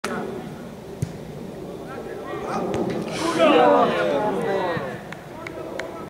11. kolejka: MKS Siemianowiczanka II 1-3 Grunwald
14. minuta: główka Kota trafia w poprzeczkę